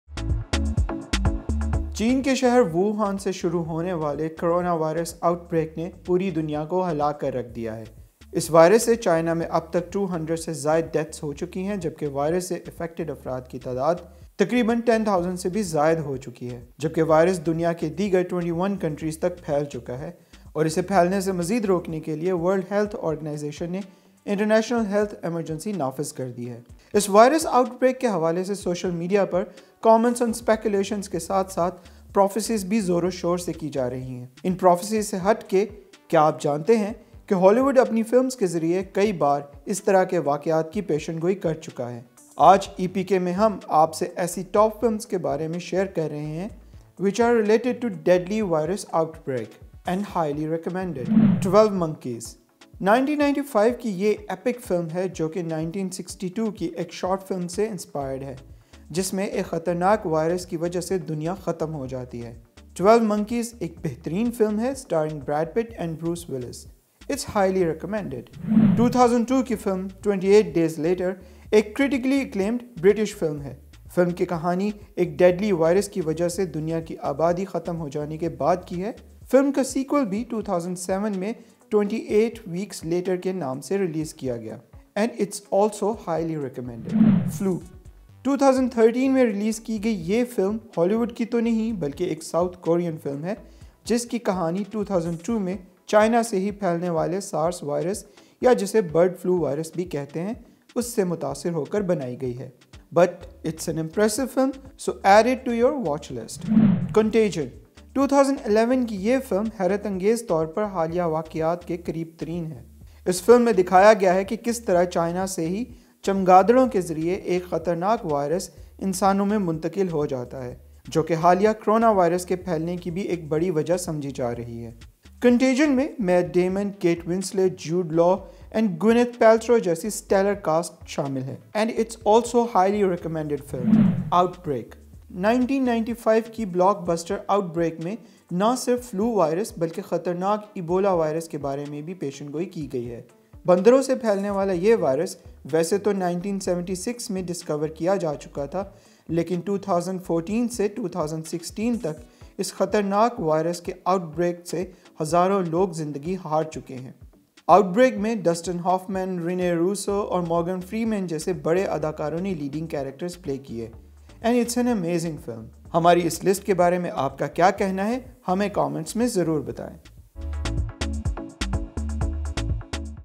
Hollywood Films Which Warned Us Way Back Of Coronavirus | Epk News | Special Report
Lot of films made in the past in Hollywood that warned us of a life threatening viruses, some films even showed situations that are pretty similar to what we saw during Carona Virus.
Epk did some searching and brings you a list of films from the past that shows the similar virus & situations similar to what we saw during this bad time.